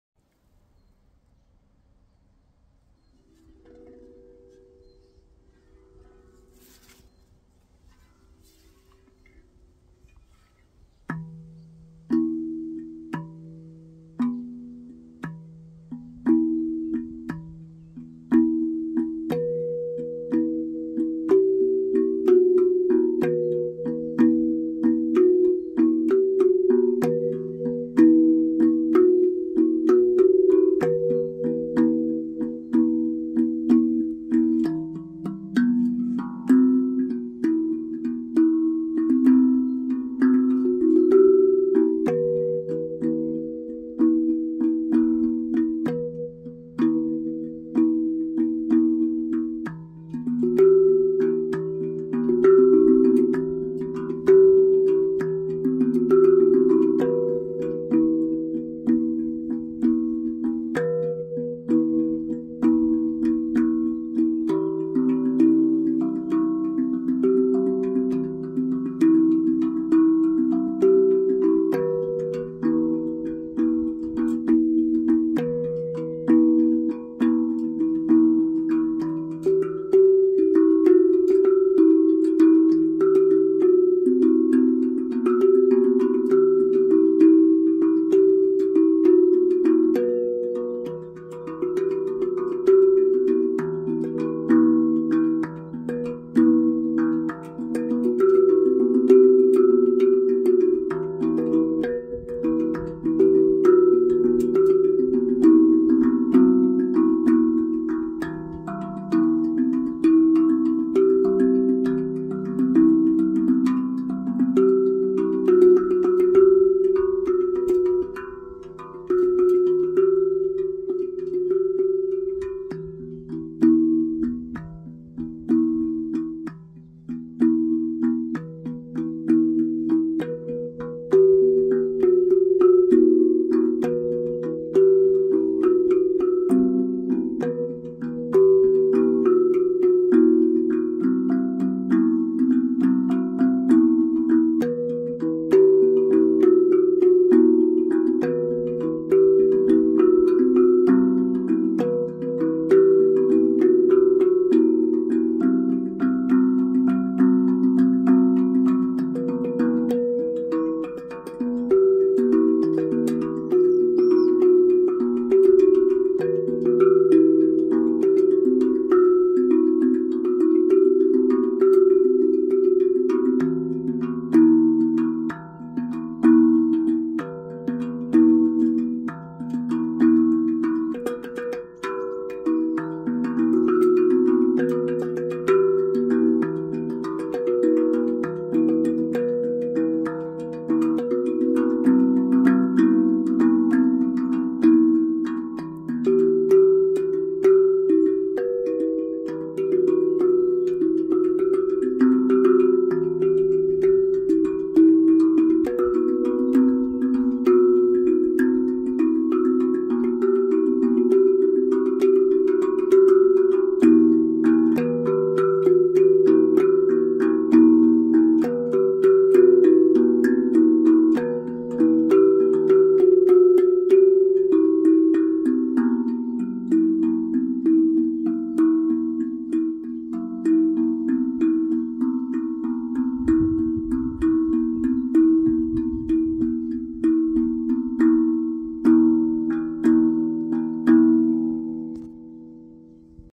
Дзен барабан глюкофон 40 см Цветок жизни
Дзен барабан Цветок жизни.

Этот инструмент имеет размер барабана 40 см ,обладает глубоким сустейном для мягкого погружения в звук.  Дзен барабан в настройке акебоно ми минор , которая на мой взгляд и слух является самой мистической , вы только вслушайтесь в эти интервалы )
 На инструменте выполнена глубокая гравировка ,Мандала, а в середину вписан Цветок жизни . Этот дзен барабан выглядит как какой-то Невероятный артефакт .